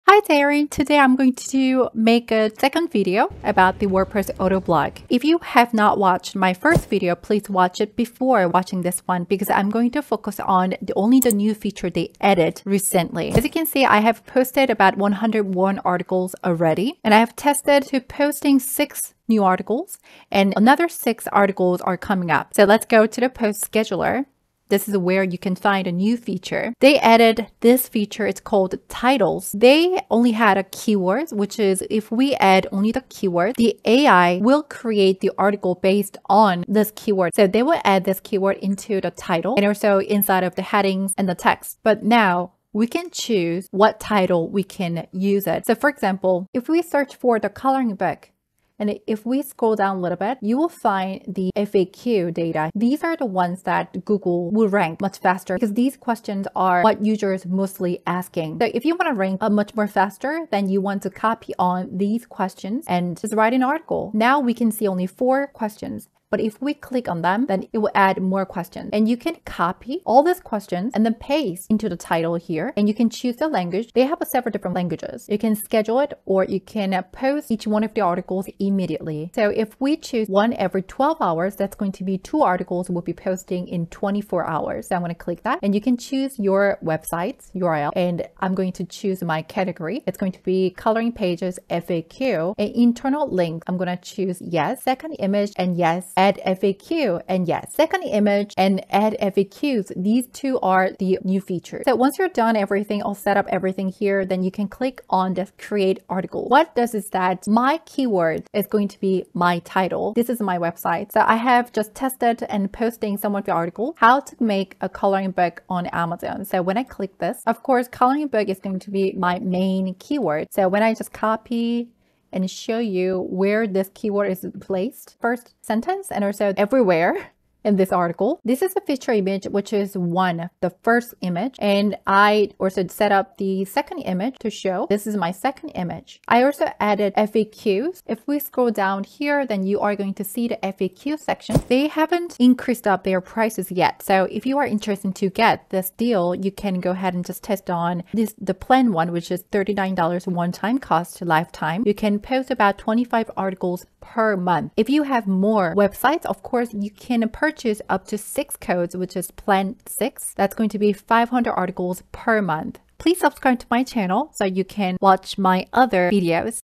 SEO ranking strategy | Auto blogging wordpress setup with WPautoblog
Auto blogging can be an effective strategy to boost your website's SEO rankings. By automatically publishing content from various sources, you can generate a steady stream of new and relevant content for your website. One popular tool for auto blogging is WPautoblog